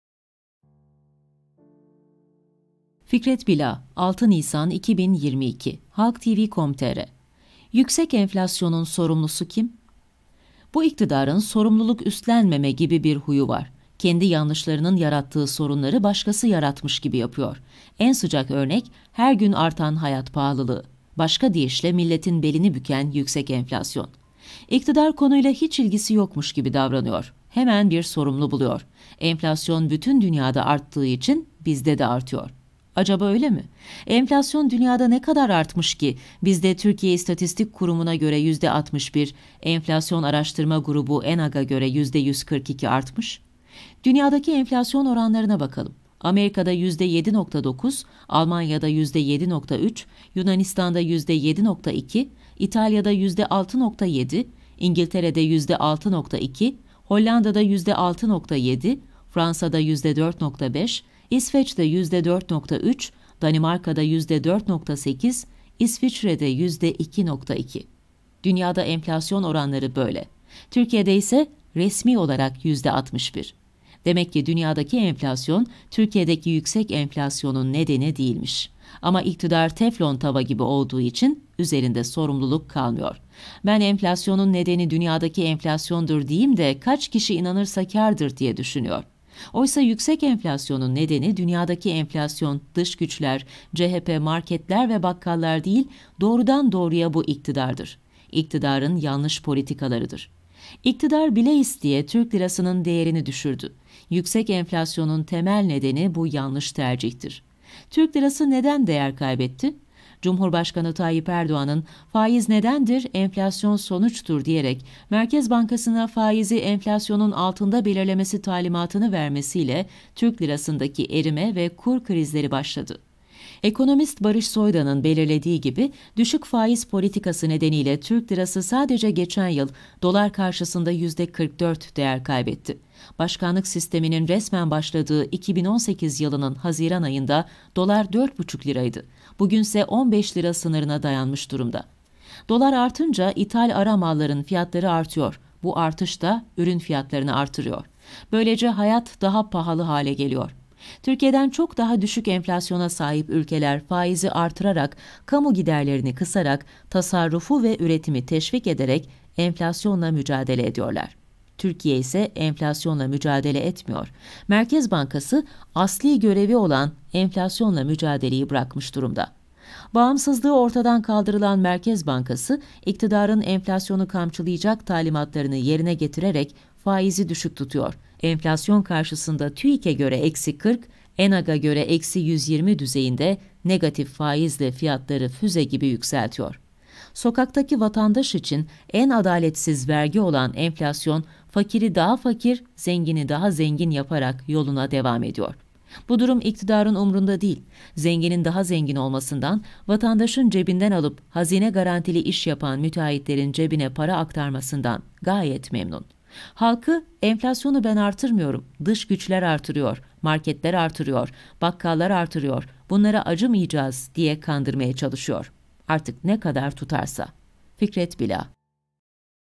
FİKRET BİLA - Yüksek Enflasyonun Sorumlusu Kim? * Sesli Köşe Yazısı Dinle *
Şu anda, ' Fikret Bila 'nın 6 Nisan 2022 tarihinde ' HalkTv.com.tr 'de yayınlanan ' köşe yazısı 'nı dinliyorsunuz.